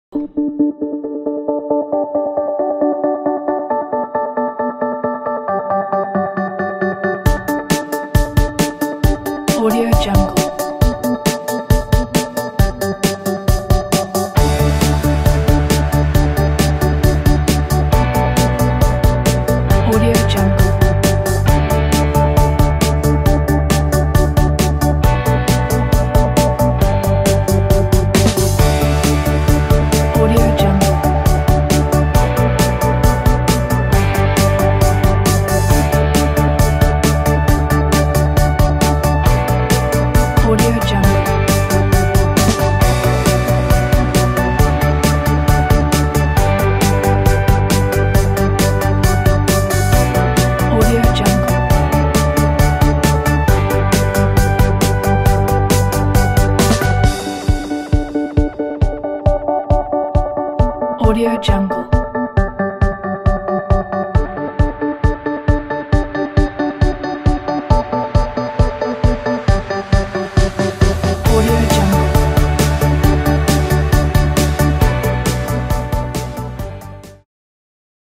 After Effects Project Files - 24 Transitions - VideoHive 9510077
24 transitions easy to use for your projects of films, teasers, documentaries, advertising and slideshows - Full HD - After Effects CS4 or higher - No Plugin required - Pdf Tutorial include - Audio... (broadcast-packages) Read more